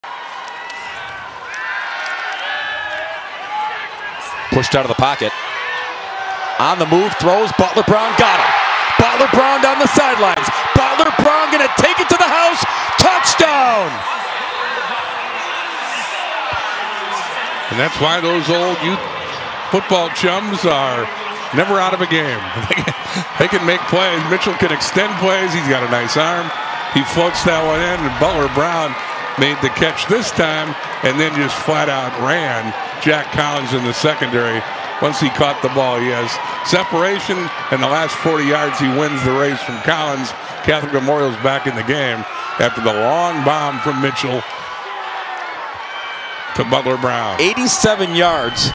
WIAA Football: 87 yd. TD
Marvanous Butler-Brown catches the MJ Mitchell pass in stride and goes 87 yards to get Memorial back in the game.